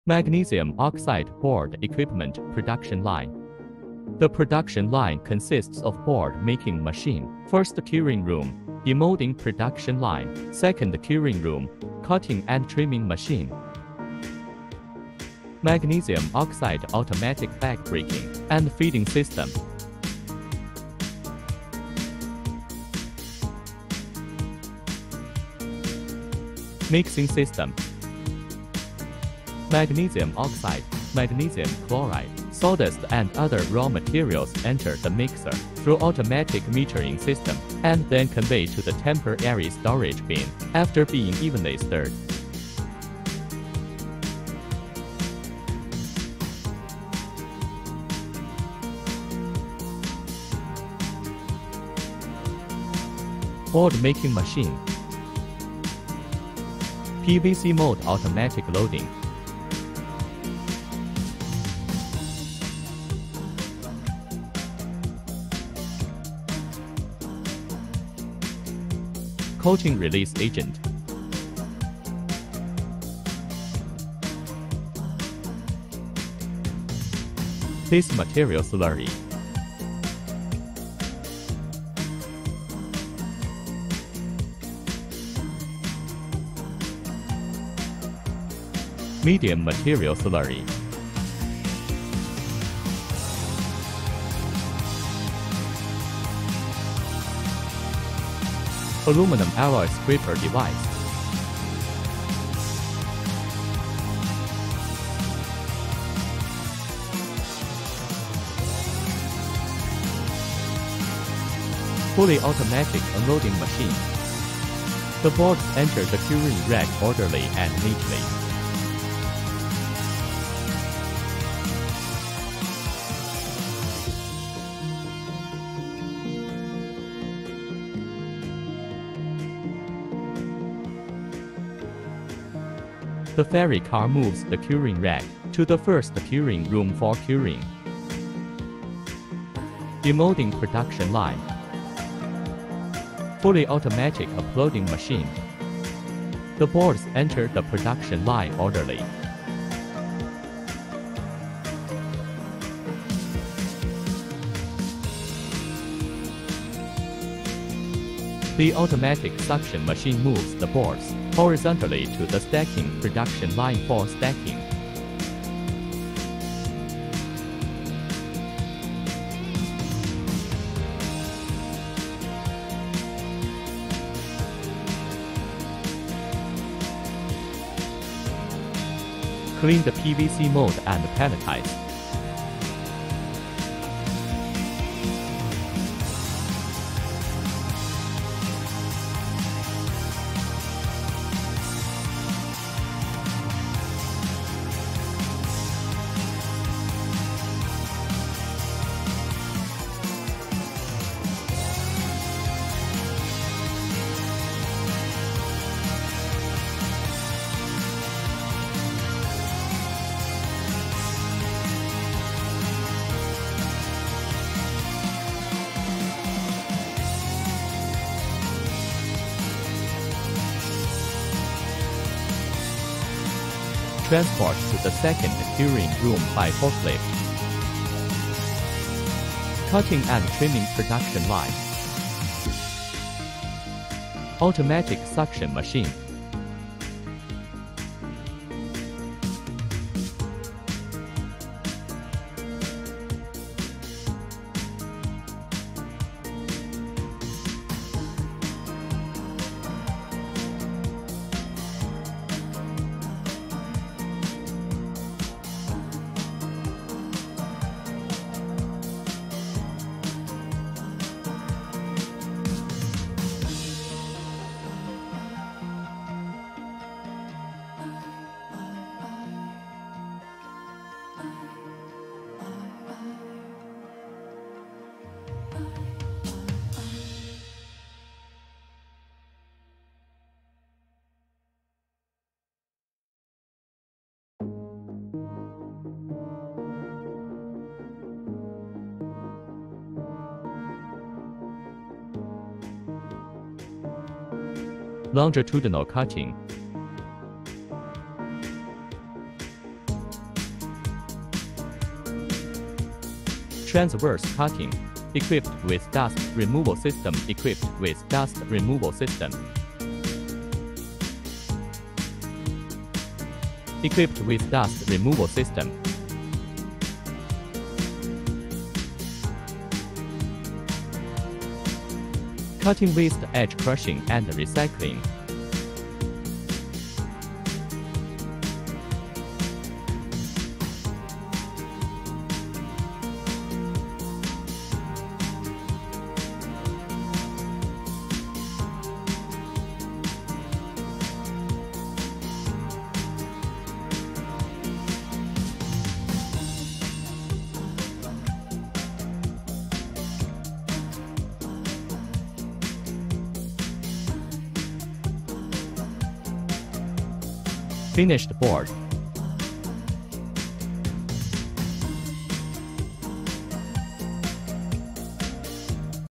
Mgo board machine｜ mgo board production line｜ mgoboard mgoboard mgo board making machine
⚙️The mgo board machine or Magnesium oxide board machine, also called mgo board production line, can produce the mgo board and MgO plane, is the professional mgo board machinery equipment for producing the new board

⚙️The manufacturing process of mgo boardl includes:

1. Magnesium oxide board is made of magnesium oxide, magnesium chloride and water as the main raw materials, with wood chips and perlite as auxiliary fillers for mixing and stirring, and glass fiber cloth and non-woven fabric as reinforcing and toughening materials. Equipment flow slurry production line
2. Raw material metering and stirring: magnesium oxide, magnesium chloride solution, sawdust, perlite, water and other raw materials are automatically metered into the mixer and stirred evenly.
3. Sheet forming: The automatic plate suction machine absorbs the PVC template and moves it to the sheet production line, applies a release agent, the fabric slurry falls on the PVC template, lays glass fiber cloth and non-woven fabric, and scrapes the slurry. The slurry and the bottom material fall into the production line in turn, lay the glass fiber cloth and non-woven fabric, scrape the slurry, and after the automatic cutting machine cuts the glass fiber cloth, the plate regularly enters the curing frame
4. One-time curing: curing room temperature is 20-35℃, curing time is 1 day
5. Demoulding: separate the PVC formwork and the sheet
6. Secondary curing: curing room temperature is 30-40℃, curing time is 3 days
7. Cutting and trimming, output finished sheet

⚙️Links⚙️
Mgo panel machine

⚙️Socials⚙️
Wechat: ChinaDeyi88